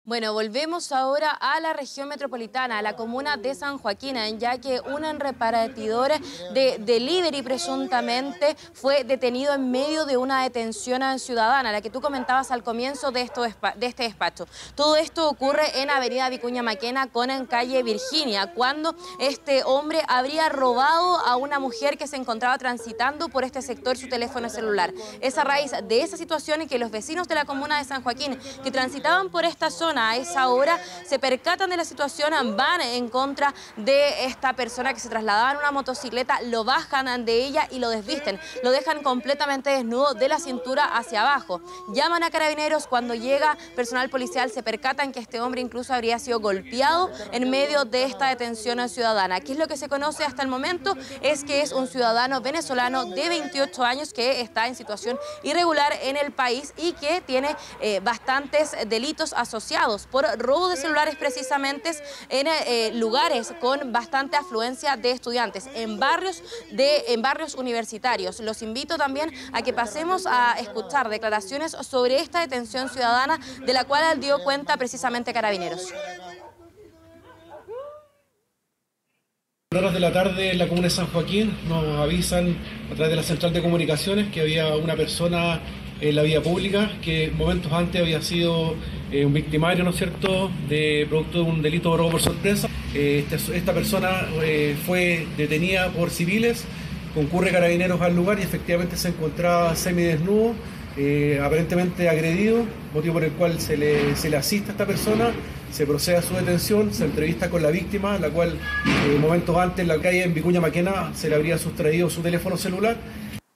Lo golpearon y desnudaron: presunto motochorro fue detenido por vecinos en San Joaquín
Los traseúntes actuaron luego de que el sujeto robó el celular a una mujer en plena calle.